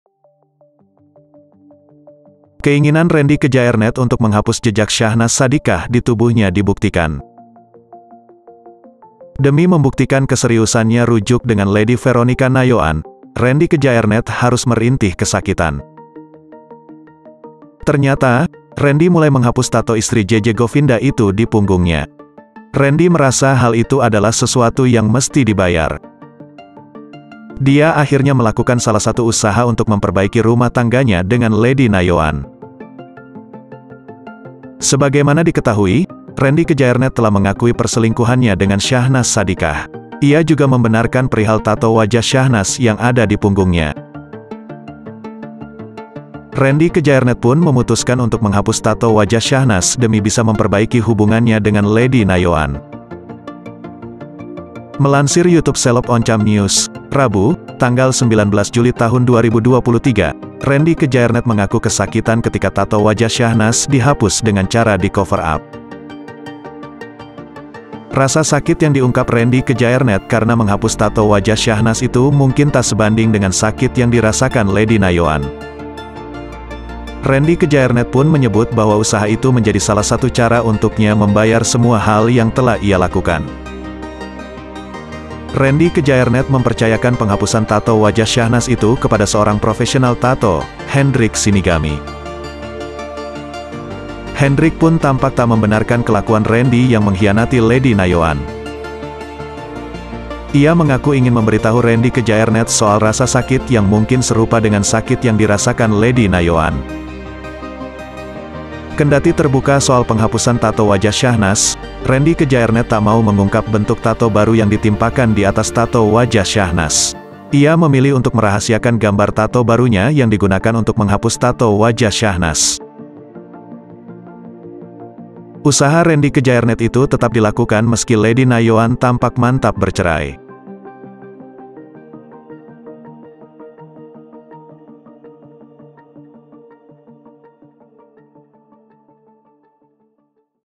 Hapus Tato Syahnaz Demi Lady Nayoan, Rendy Kjaernett Merintih Kesakitan
Keinginan Rendy Kjaernett untuk menghapus jejak Syahnaz Sadiqah di tubuhnya dibuktikan.

Demi membuktikan keseriuasannya rujuk dengan Lady Veronica Nayoan, Rendy Kjaernett harus merintih kesakitan.

Ternyata, Rendy mulai menghapus tato istri Jeje Govinda itu di punggungnya.

Rendy merasa hal itu adalah sesuatu yang mesti dibayar.